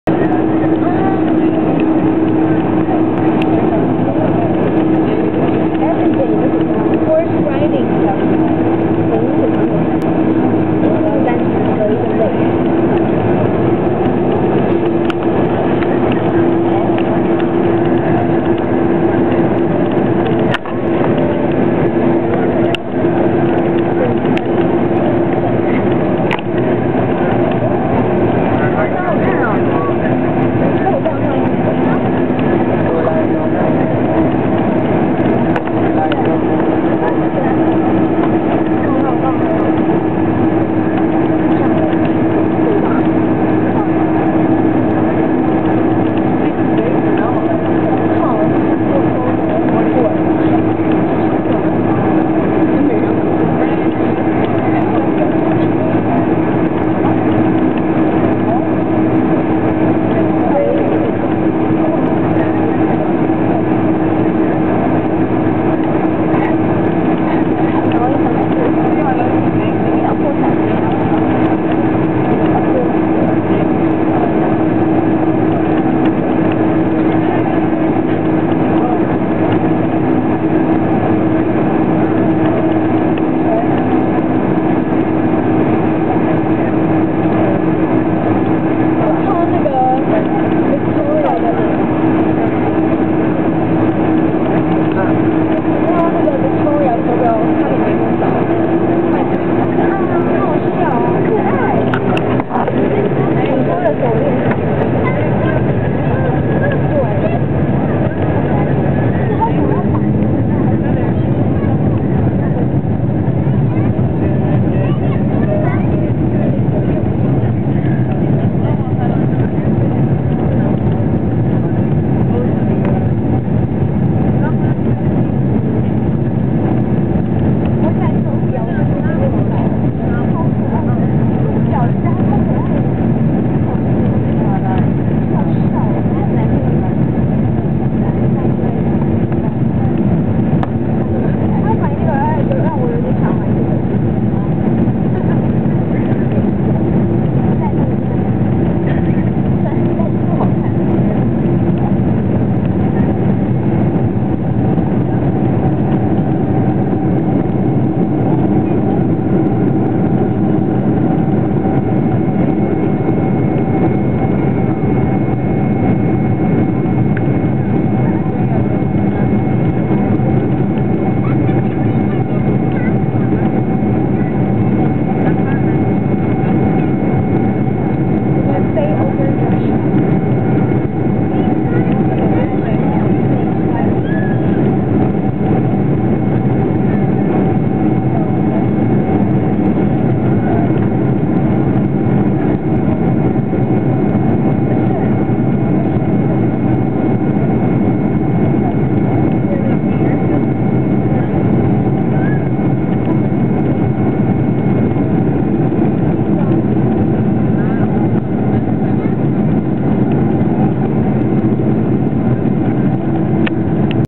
KLM Boeing 747-400 takeoff at Amsterdam Schiphol
KLM Boeing 747-400 takeoff at Amesterdam Schiphol boud to Bangkok BKK Thailand